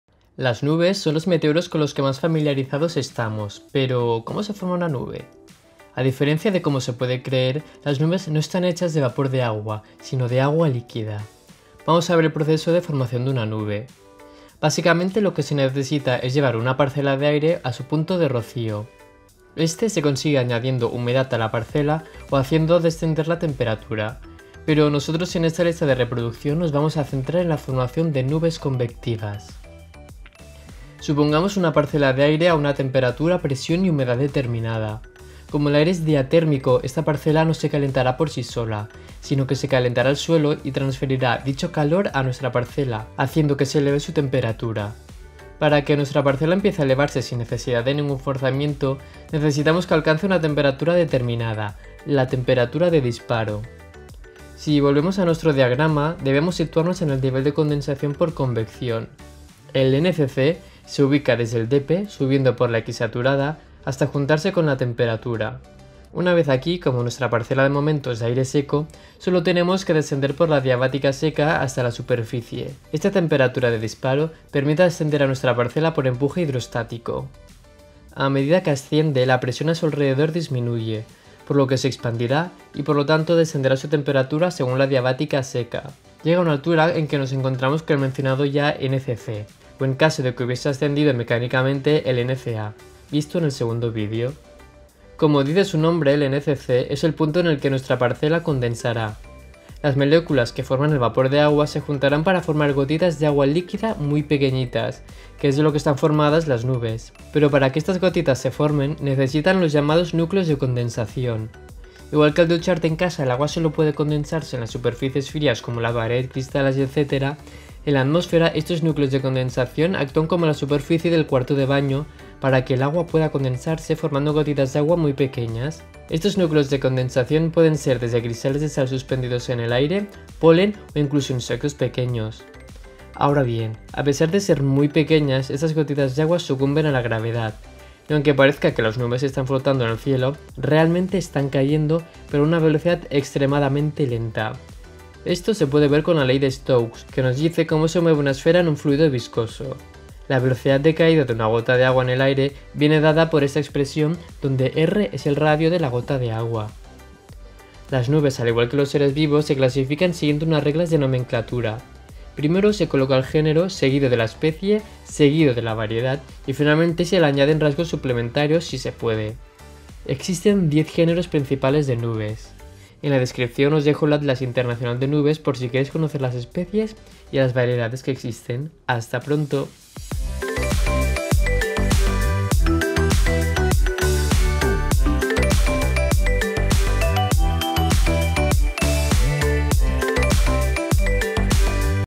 ¿Cómo se forman las nubes?
Alguna vez te has preguntado de qué están hechas y cómo se forman las nubes? Al contrario de lo que se suele pensar, las nubes no están hechas de vapor de agua, si no no se verían. Están formadas por agua líquida!
Es recomendable ver los dos primeros vídeos.

Guión creado por: Joan Tisdale
Animaciones creadas por: Joan Tisdale
Voz en Off: Joan Tisdale
Animaciones creadas con: Adobe After Effects

Secciones:
00:00 Introducción
00:14 Formación de las nubes
01:46 Nucleación
02:13 Ley de Stokes
02:37 Clasificación de las nubes